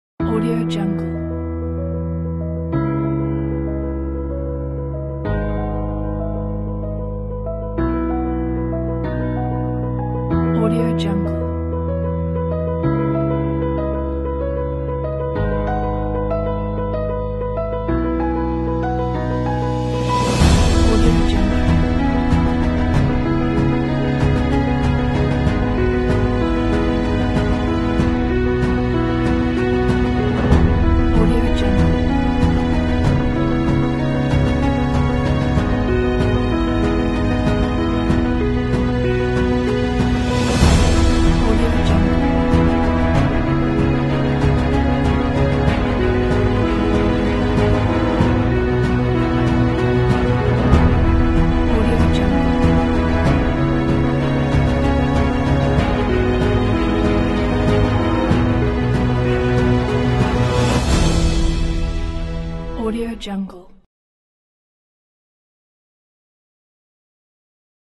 Winter Photo Gallery | After Efects Project Files
1920×1080 full HD
1 min 07 sec duration
12 Photo/Video placeholders
13 Title placeholders
Fonts, Photos and Music not included